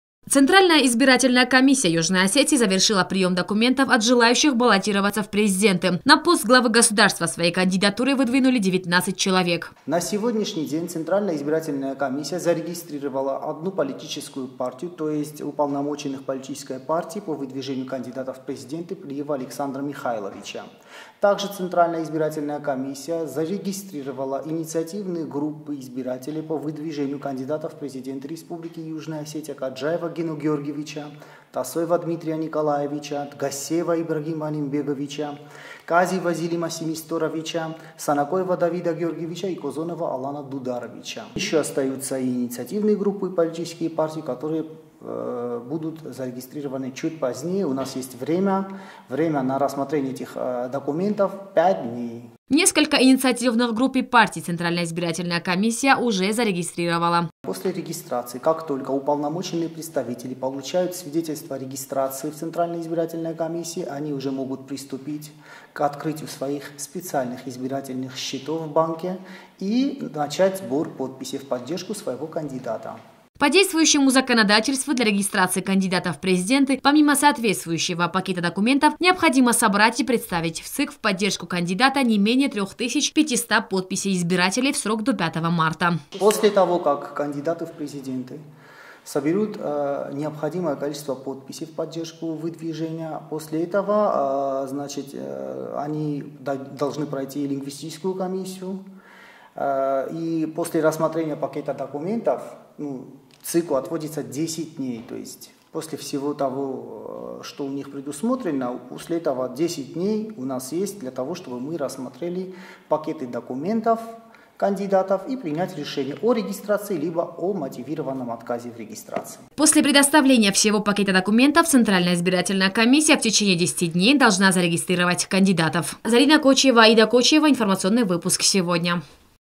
ЦИК республики завершил прием документов кандидатур, баллотирующихся на пост президента Южной Осетии
ЦИК республики в воскресенье, 20-го февраля завершил прием документов кандидатур, баллотирующихся на пост президента Южной Осетии. Пакеты документов на регистрацию инициативных групп и представителей кандидатов подали 19 заявивших о себе кандидатов в президенты - 15 выдвиженцев от инициативных групп и четыре - от политических партий.